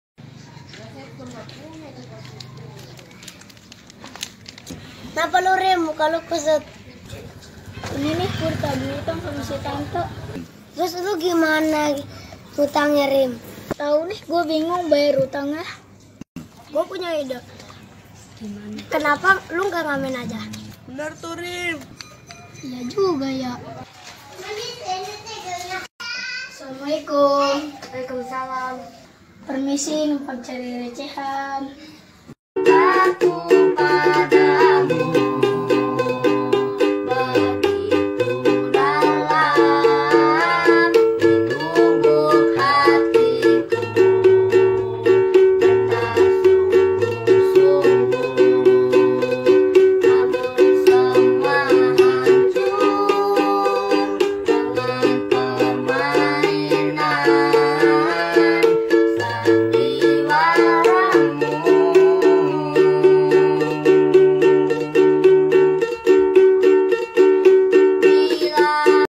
film pendek arim ngamen untuk bayar hutang episode 6
Film arim adalah konten hiburan untuk anak-anak arim mencoba untuk mengolah vocalnya (ngamen) untuk membayar hutang-hutangnya di warung tante.arim anak yang tidak mudah menyerah pekerjaan apapun ia lakukan asalkan hallal.